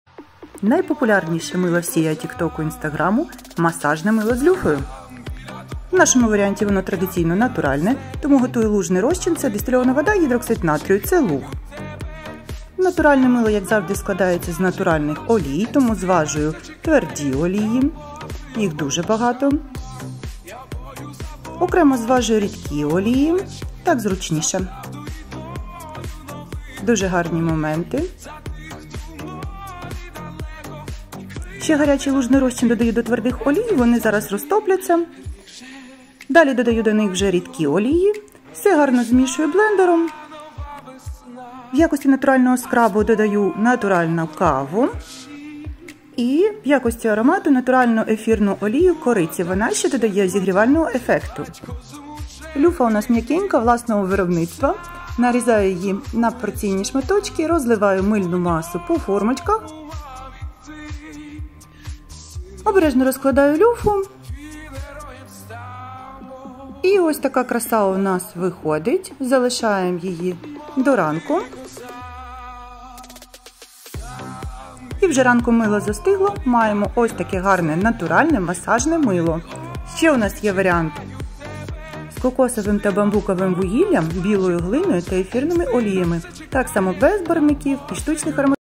Масажне мило з кавою, люфою та ефірною олією кориці
Найпопулярніше мило всього інстаграму та тік-току. В нашому варіанті воно, традиційно, натуральне, без барвників та штучних ароматизаторів.